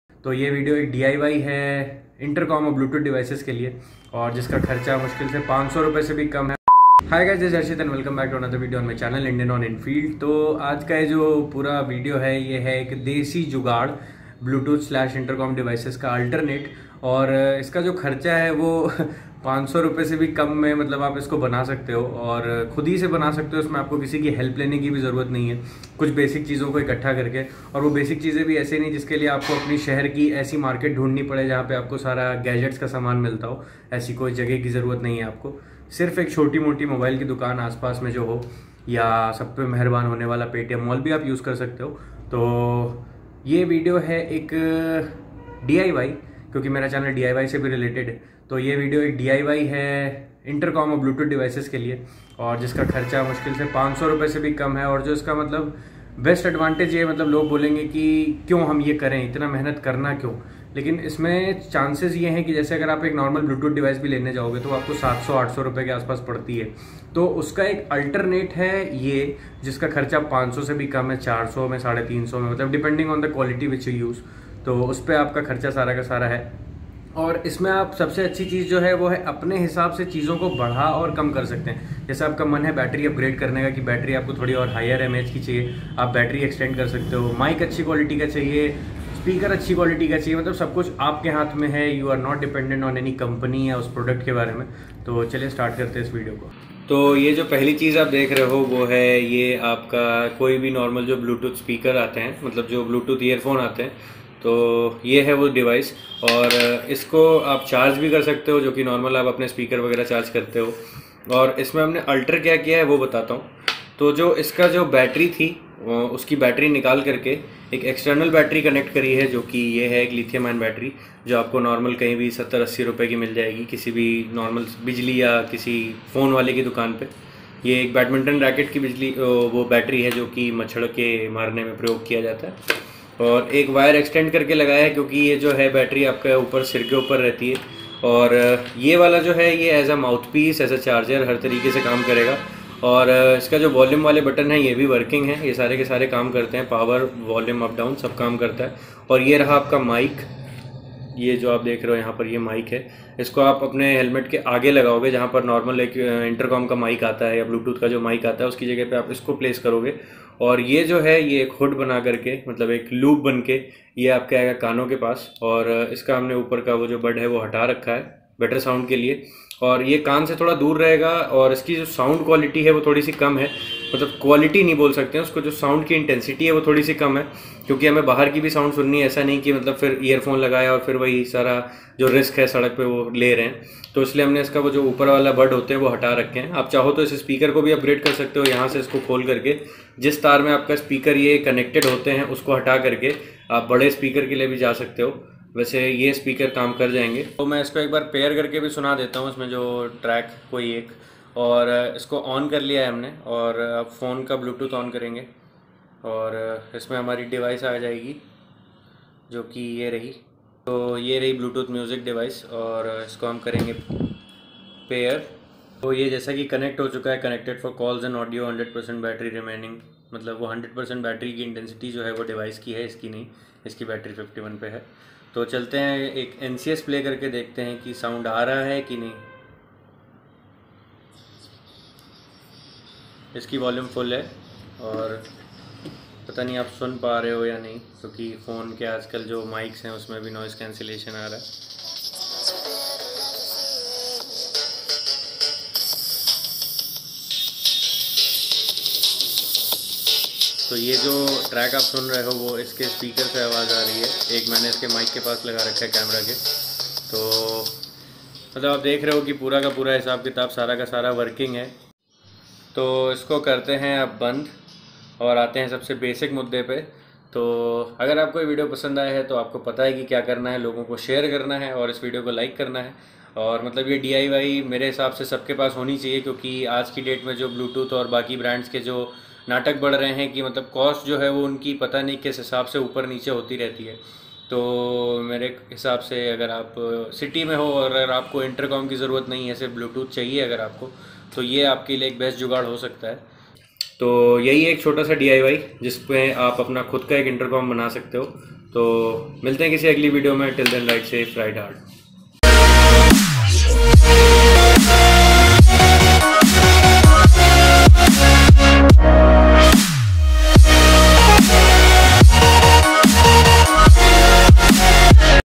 DIY HELMET INTERCOM IN LESS THAN 300 ₹ | Turn Any Helmet Into A Wireless Bluetooth Helmet
THINGS YOU WILL NEED:-
BLUETOOTH HEADSET
AND ANY BATTERY WHICH YOU WILL GET IN LOCAL MARKET MORE CHEAP thats it you are good to go
GADGETS WHICH I USE:-
Primary Camera
Secondary Camera
Lenses Which I Use
50mm ( A MUST HAVE LENS )
MANORANJAN BHI JAROORI HAI KAAM KE SAATH SAATH